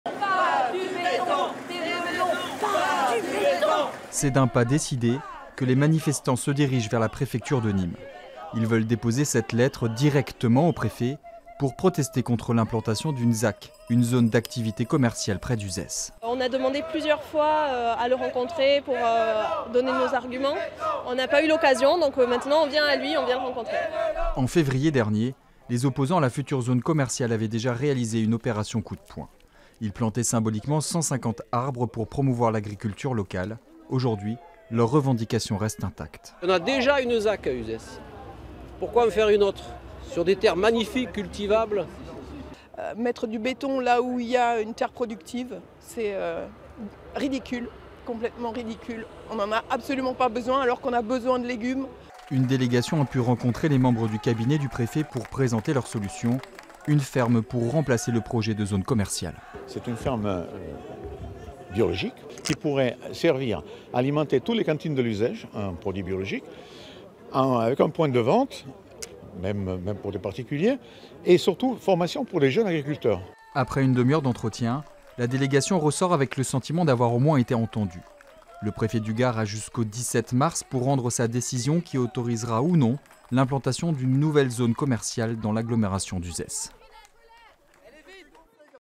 Nîmes : des manifestants défilent contre le projet de la ZAC des Sablas
Près de cinquante manifestants se sont donnés rendez-vous à Nîmes pour manifester contre le projet de la future ZAC des Sablas à Montaren et Saint Médiers.